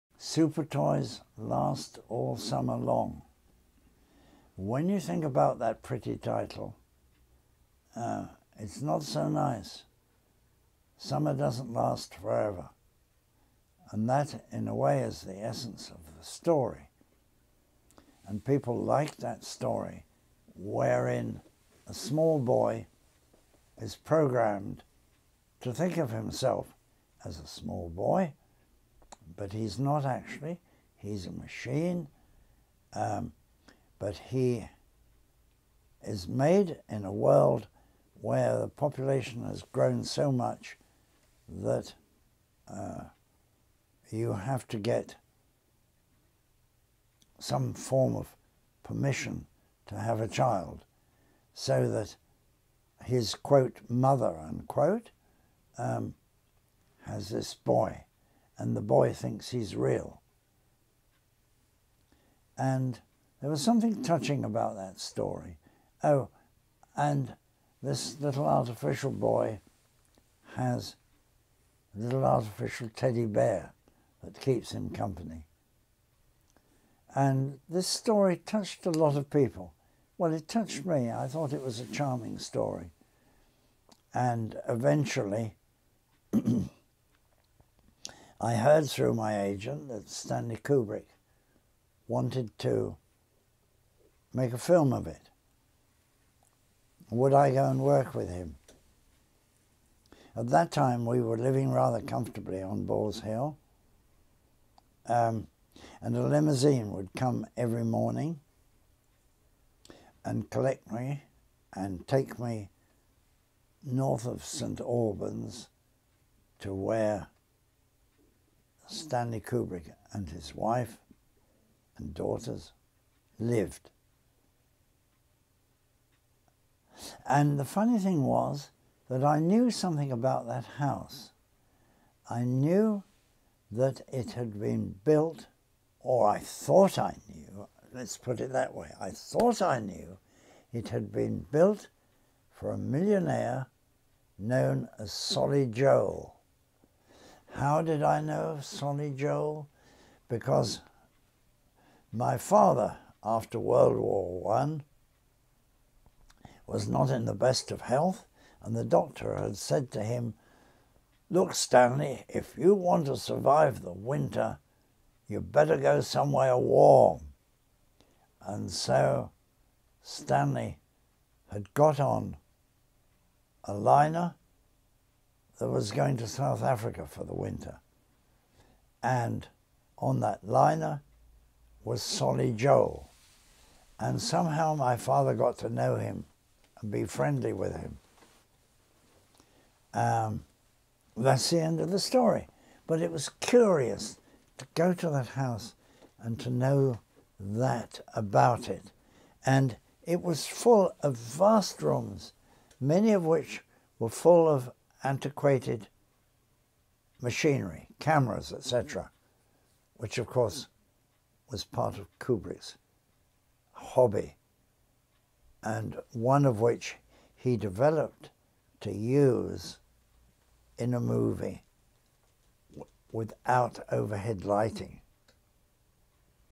Brian Aldiss - Stanley Kubrick’s house (67/79)
Writer Brian Aldiss (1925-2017) was best known for his science fiction novels and short stories. 'Non-Stop', his first science fiction novel, was published in 1958. His many award-winning titles include 'Hothouse', 'The Saliva Tree' and 'Helliconia Spring'. [Listener: Christopher Sykes; date recorded: 2014]

TRANSCRIPT: Supertoys Last All Summer Long. When you think about that pretty title, it's not so nice.

Summer doesn't last forever, and that, in a way, is the essence of the story. And people liked that story wherein a small boy is programmed to think of himself as a small boy, but he's not actually, he's a machine. But, he is made in a world where the population has grown so much that you have to get some form of permission to have a child so that his 'mother' has this boy and the boy thinks he's real. There was something touching about that story – oh, and this little artificial boy has a little artificial teddy bear that keeps him company. And this story touched a lot of people... well, it touched me. I thought it was a charming story. And eventually I heard through my agent, that Stanley Kubrick wanted to make a film of it. Would I go and work with him?

At that time, we were living rather comfortably on Boar's Hill, and a limousine would come every morning and collect me and take me north of St Albans to where Stanley Kubrick and his wife and daughters lived. And the funny thing was that I knew something about that house. I knew that it had been built – or I thought I knew, let's put it that way – I thought I knew that it had been built by a millionaire known as Solly Joel. How did I know of Solly Joel? Because my father, after World War I, was not in the best of health and the doctor had said to him, 'Look, Stanley, if you want to survive the winter, you better go somewhere warm'. And so Stanley had got on a liner that was going to South Africa for the winter, and on that liner was Solly Joel. And somehow my father got to know him and be friendly with him.

That's the end of the story, but it was curious to go to that house and to know that about it. And it was full of vast rooms, many of which were full of antiquated machinery: cameras, etc. which, of course, was part of Kubrick's hobby, and one of which he developed to use in a movie without overhead lighting.